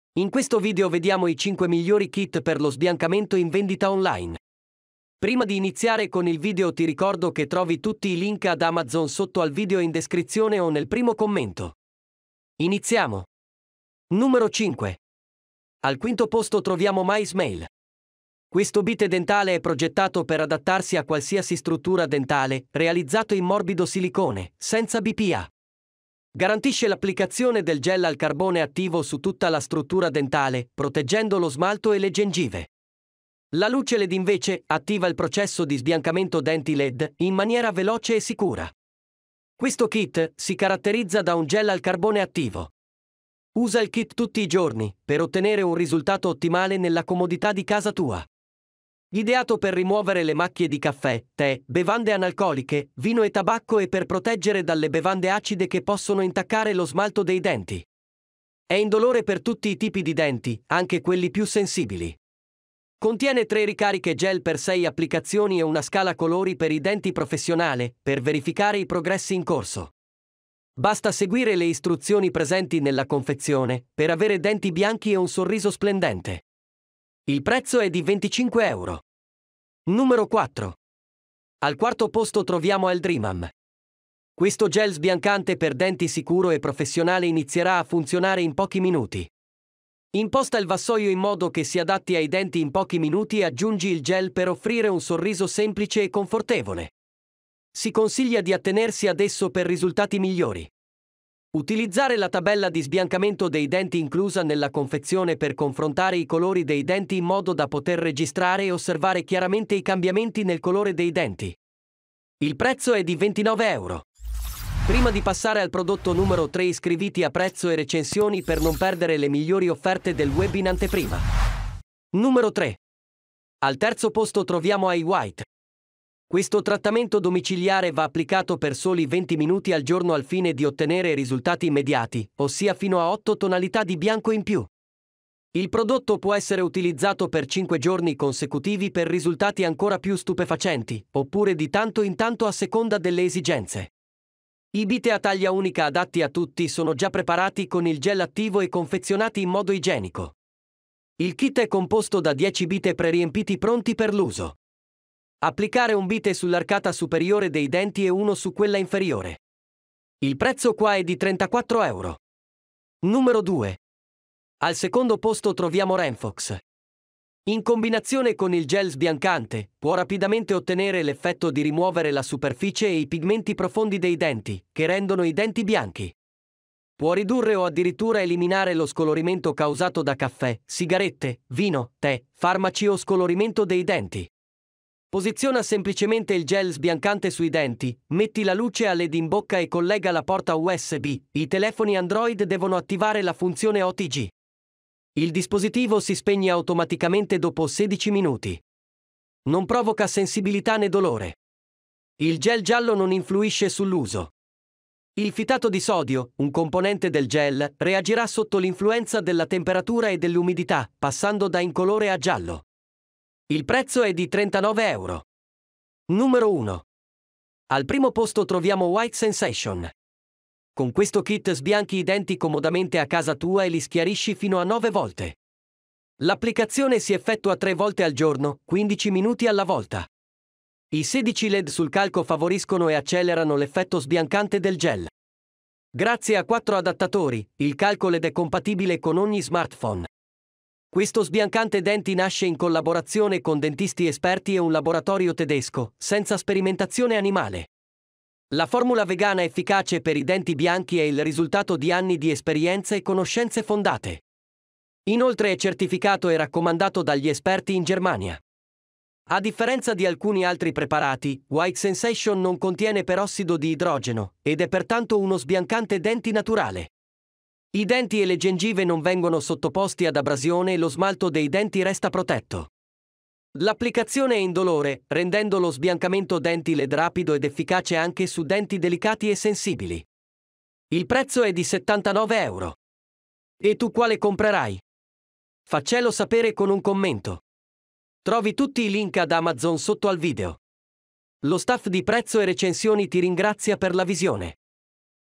Kit Sbiancamento Denti - I 5 Migliori (2022)
5) MySmile Kit Sbiancamento Denti

In questo video vediamo i 5 migliori kit di sbiancamento denti in vendita online.